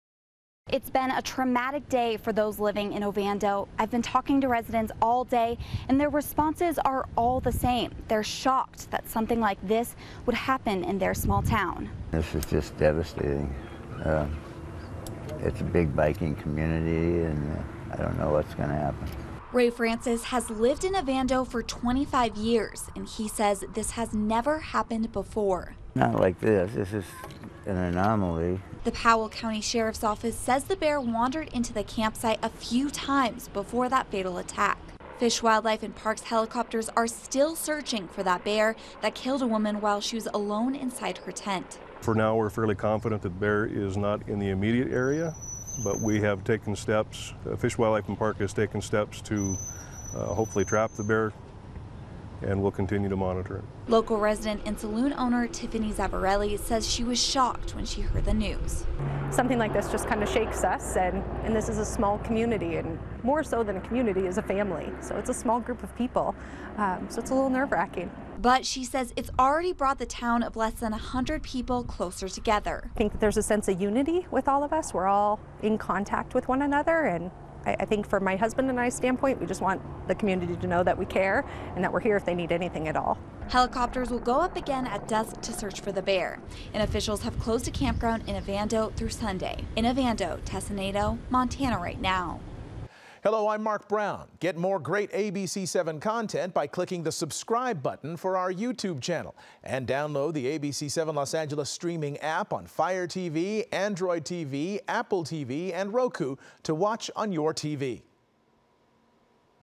Bear pulls CA cyclist out of her tent while she was camping in Montana, kills her| ABC7
A 400-pound bear pulled a woman from her tent in the middle of the night and killed her before fellow campers used bear spray to force the grizzly out of the area. Wildlife officials are still trying to capture the animal.